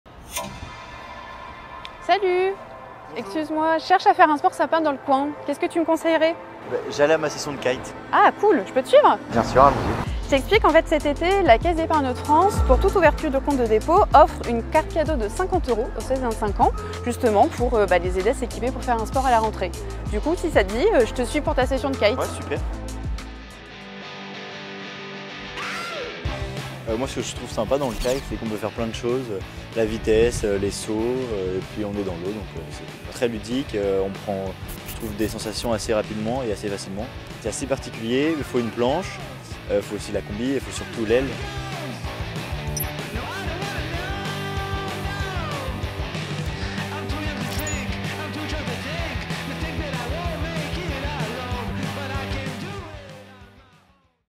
Offre Jeunes Sport : Kitesurf
On se jette à l’eau et on se met au sport avec la Caisse d’Épargne Hauts-de-France ! Pour toute ouverture d’un compte de dépôt 16-25 ans, on vous offre une carte cadeau de 50€ valable dans un magasin de sport.
Conditions et liste des enseignes participantes sur caisse-epargne.fr

*Pour toute nouvelle ouverture définitive d’un compte de dépôt par une personne âgée de 16 à 25 ans (l’ouverture à un mineur est soumise à autorisation des représentants légaux). Sous réserve d’acceptation de votre dossier par la Caisse d’Epargne Hauts de France. Offre valable du 30/05/2023 au 02/09/2023 et dans la limite des 1000 premiers souscripteurs d’un compte de dépôt. Loisichèque est un produit de la société SDV-GROUPE CINECHEQUE - RCS Versailles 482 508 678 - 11 rue Camille BLANC - 78240 CHAMBOURCY - Pour plus d’informations (coût de connexion selon votre fournisseur d’accès). Capital social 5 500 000€.